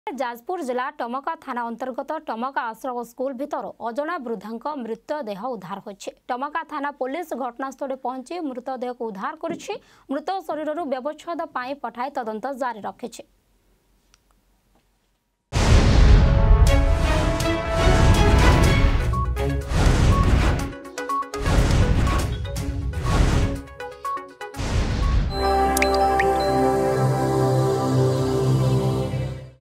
ସ୍କୁଲ ଭିତରୁ ଅଜଣା ବୃଦ୍ଧାଙ୍କ ମୃତ୍ୟୁ ଦେହ ଉଦ୍ଧାର
ଯାଜପୁର ଜିଲ୍ଲା ଟମକା ଥାନା ଅନ୍ତର୍ଗତ ଟମକା ଆଶ୍ରମ ସ୍କୁଲ ଭିତରୁ ଅଜଣା ବୃଦ୍ଧାଙ୍କ ମୃତ୍ୟୁ ଦେହ ଉଦ୍ଧାର ହୋଇଛି। ଟମକା ଥାନା ପୋଲିସ ଘଟଣା ସ୍ଥଳରେ ପହଞ୍ଚି ମୃତ ଦେହକୁ ଉଦ୍ଧାର କରିଛି.
ଅଧିକ ଖବର ପାଇଁ ଡ଼ାଉନଲୋଡ କରନ୍ତୁ “Argus News” ଆପ୍